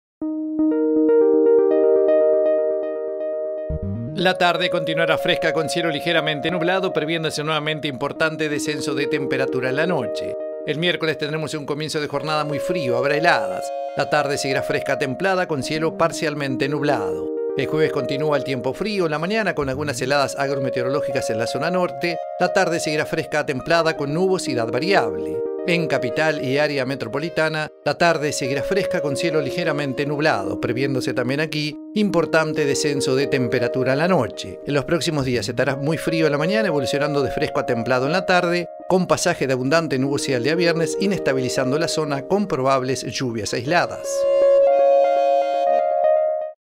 El tiempo (26/06/18)
Pronóstico del tiempo del 26 de junio de 2018, realizado por Nubel Cisneros para Televisión Nacional.